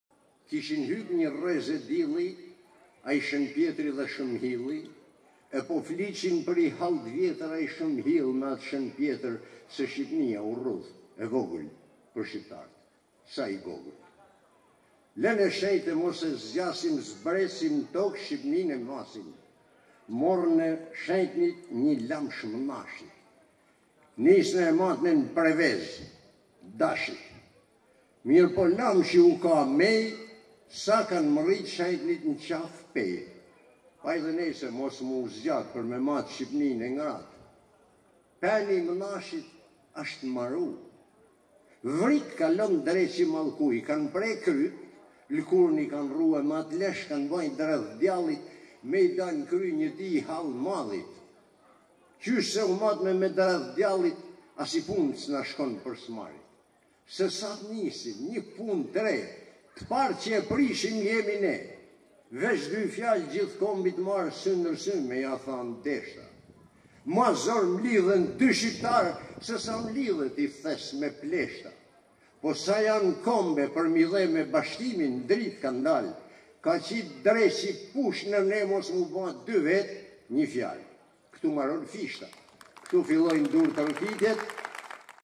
Gjergj Fishta - Recitim nga Agim Doci
GJERGJ FISHTA

Kishin hyp nji rreze dilli,
Ai Shën Pjetri e ai Shën Mhilli,
E po flisnin për i hall t'vjetër,
Ai Shën Mhill me njat Shën Pjetër

Se Shqypnia u rrudh e vogël,
Për shqyptart, sa një gogël.
Lene t'shejt e mos ta zgjasim,
Zbresin n'tokë Shqypnin ta masin.

Morën tshejt nji lamsh t'mashit
Nisen matjen n'Preveze t'tashit.
Mirëpo lamshi u ka mej,
Sa arritën t'Shejtit n'Qafë-Pejë.

Edhe nejse, mos me u zgjat,
Si me mat Shqypnin e ngratë,
Peni i mashit asht mbaru...
Vrik, kalon dreqi i mallku!

I kanë pre kryet, lekurën i kanë rru,
E me atë lesh kanë ba nji dredhë t'Djallit.
Qysh se u mat me lesh t'Djallit,
Asnji punë s'na shkon për s'marit.

Sesa nisim nji pune t're,
Të parët që e prishim, jemi ne!

Troç, dy fjalë, kombit mbarë,
Sy me sy me ja thanë deshta:
Ma zor mblidhen dy shqyptarë,
Sesa mblidhet nji thes me pleshta.